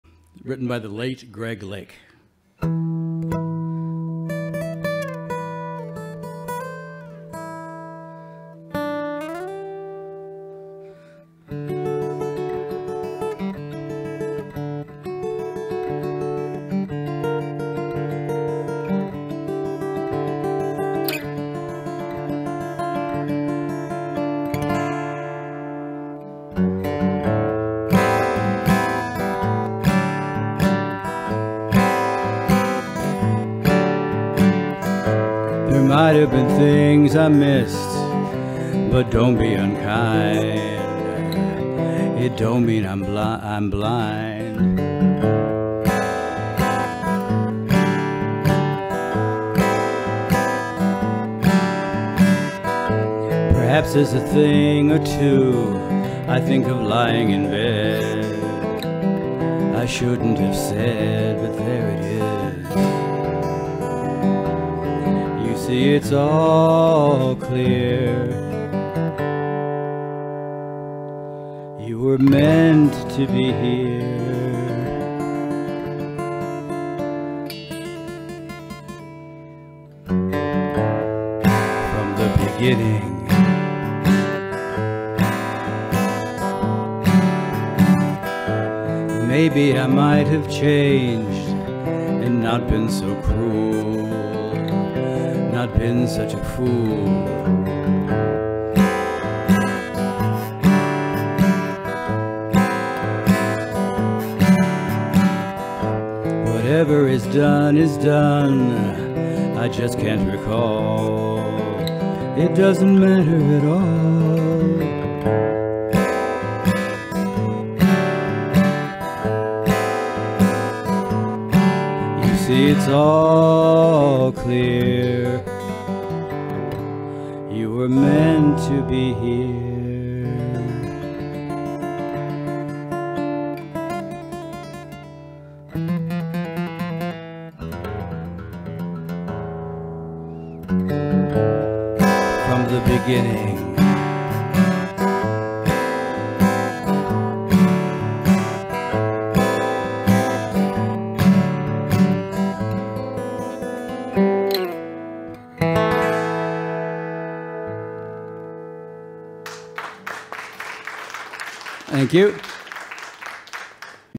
Dale Osborn 01 at Open Stage Gang Live 03 05 24
Dale Osborn performed at Trinity House Open Stage Gang - Live on March 5, 2024. This was his first song of the evening.